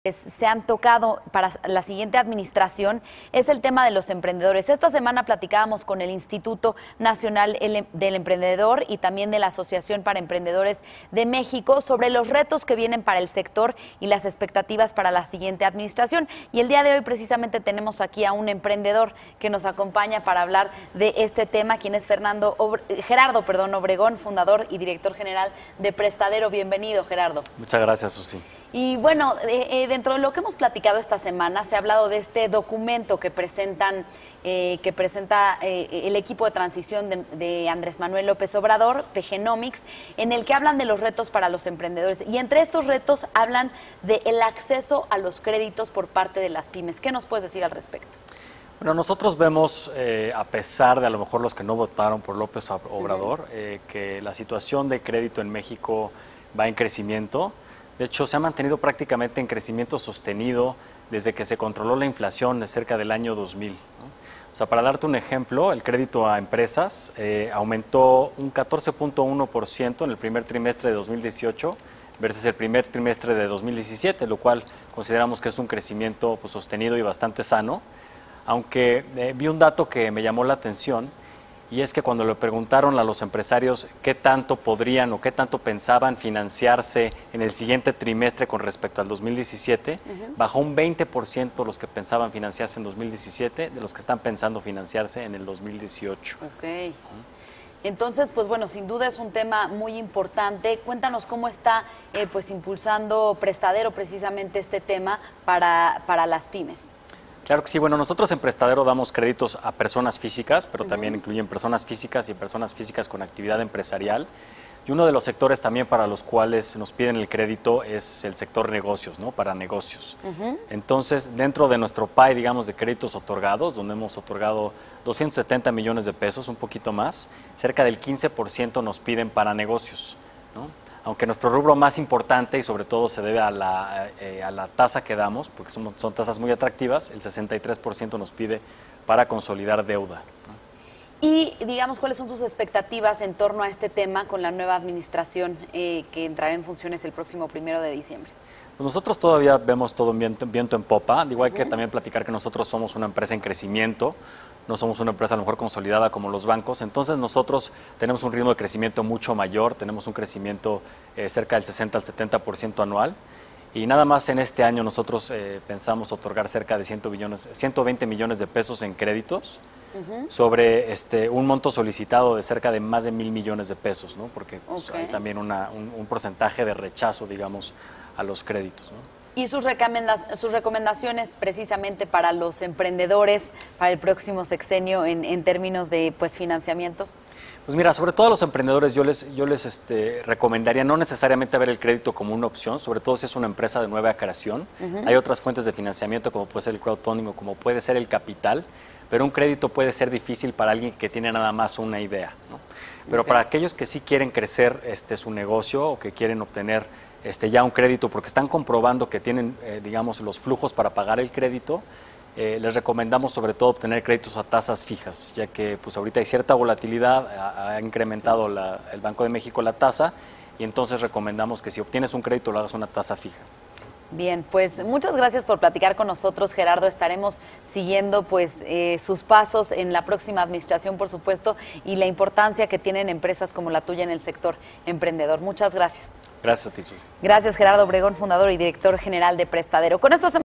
Recomendaciones para PyMEs ante la llegada de AMLO
Gerardo Obregón, fundador y director general de Prestadero sobre créditos para las PyMEs y recomendaciones para los emprendedores ante la llegada de AMLO a la Presidencia de México